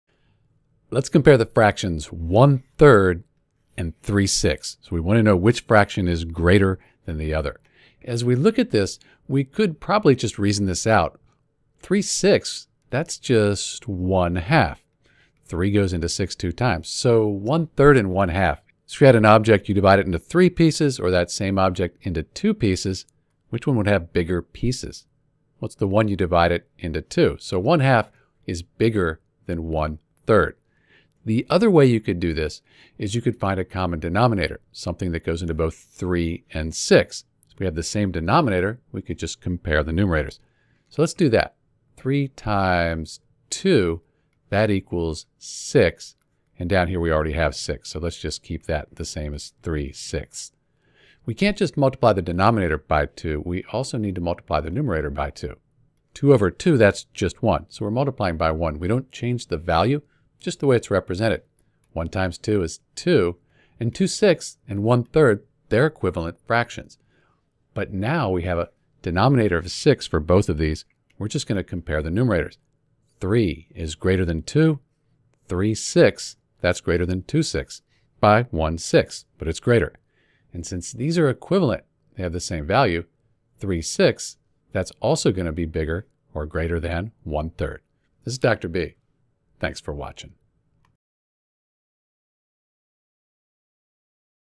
Which fraction is greater 1/3 or 3/6 ?
Second, we could also divide 1 by 3 and get approximately 0.3333. Then we divide 3 by 6 and we get 0.5. This also allows us to see that 3/6 is a larger fraction than 1/3.